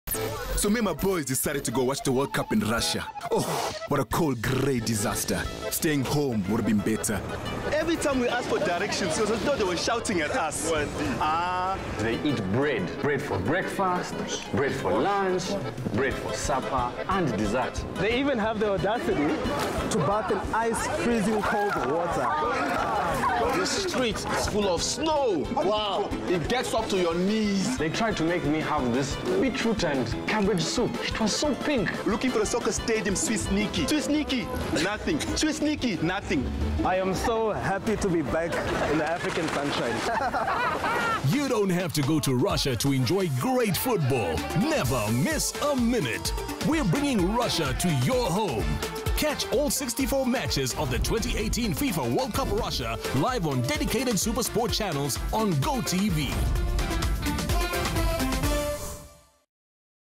GOtv FIFA World Cup Russia 2018
We're bringing Russia to your home. Catch all 64 FIFA World Cup matches live on dedicated SuperSport channels.